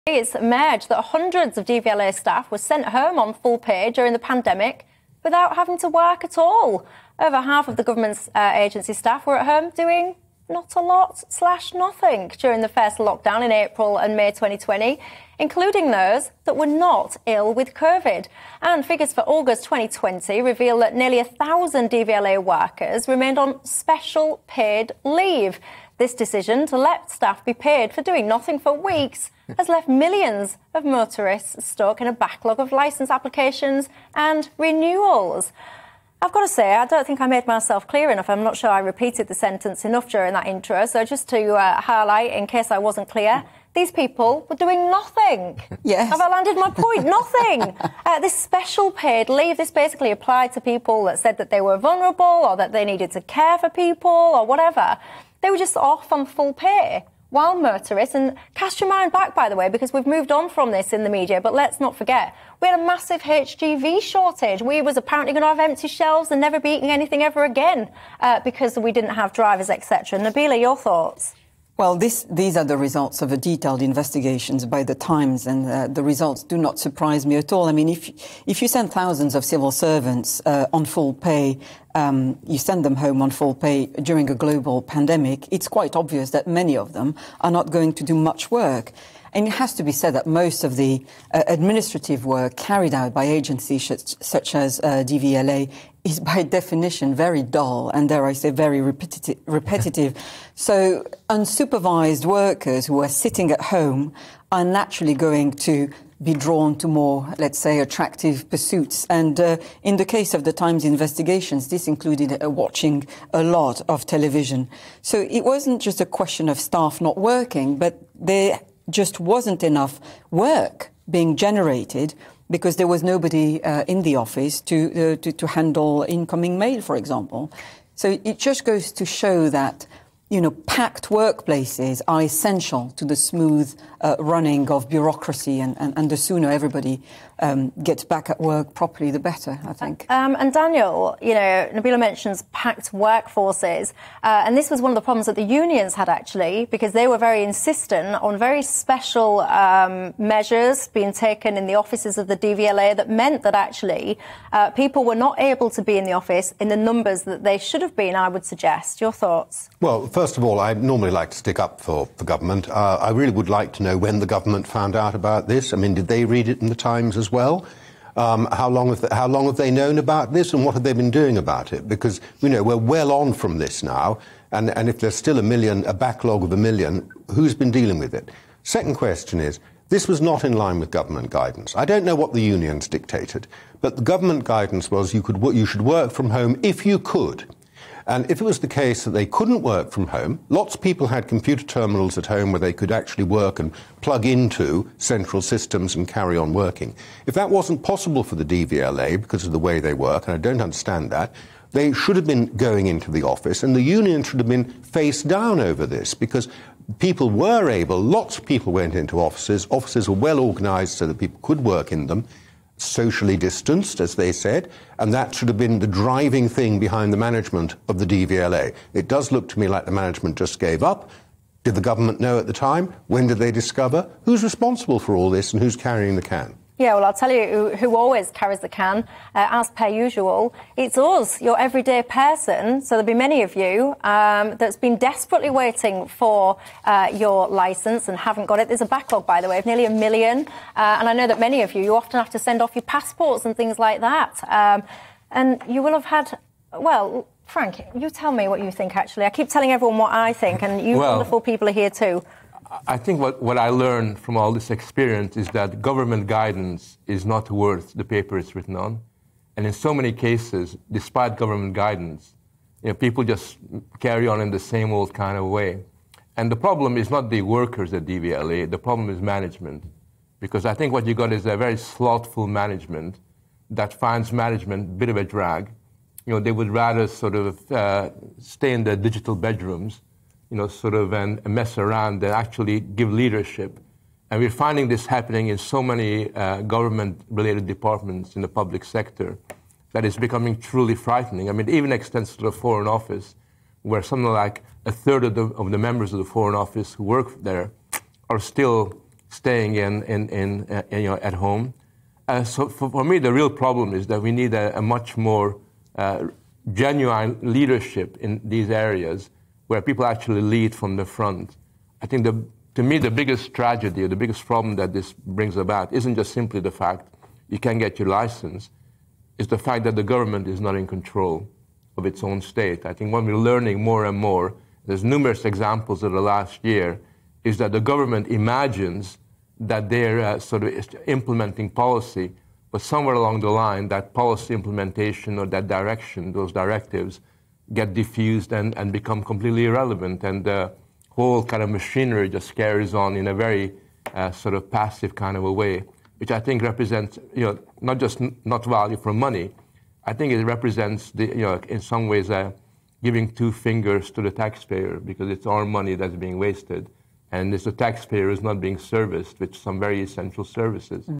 DVLA: Hundreds of staff sent home on full pay during pandemic without having to work
'We had a massive HGV driver shortage, we were apparently going to have empty shelves'

Michelle Dewberry responds to reports that hundreds of DVLA staff were sent home on full pay during the pandemic without having to work.